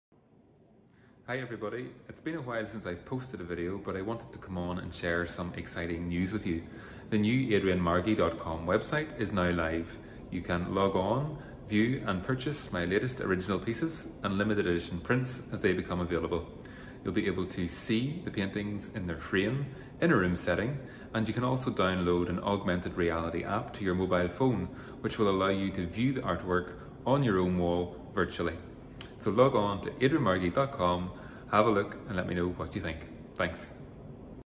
Announcing New Mobile Friendly Website & Online Shopping Experience
Delighted to officially launch the new website and online shopping experience! You can now view and purchase my latest originals and limited edition prints online.  See the artworks in a frame, in a room setting and download an augmented reality app onto your mobile phone to view my paintings on your own wall… virtually! Check it out!